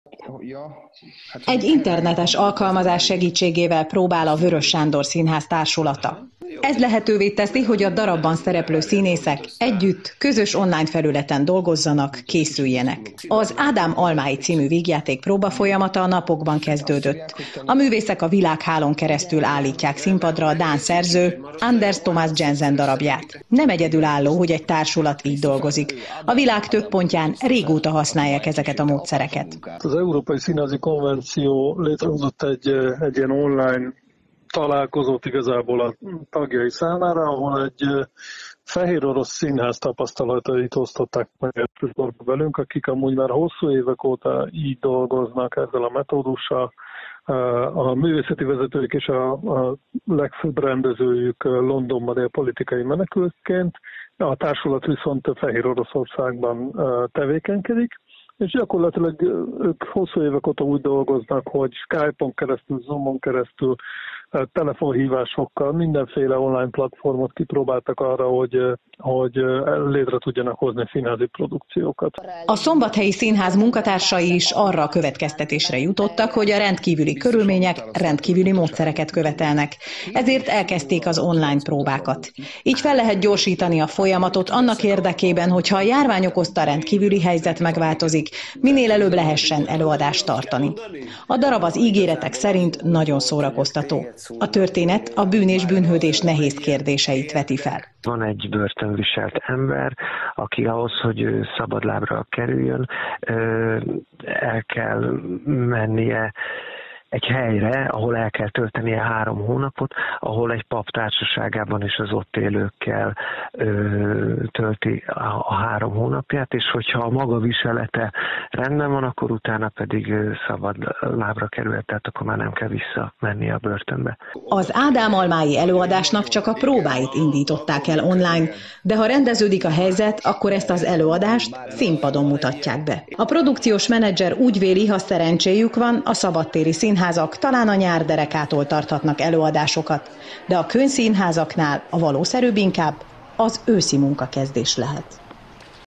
Egy internetes alkalmazás segítségével próbál a Weöres Sándor Színház társulata
Ez lehetővé teszi, hogy a darabban szereplő színészek együtt, közös online felületen dolgozzanak, készüljenek. Az Ádám almái című vígjáték próbafolyamata a napokban kezdődött, a művészek a világhálón keresztül állítják színpadra a dán szerző, Anders Thomas Jensen darabját.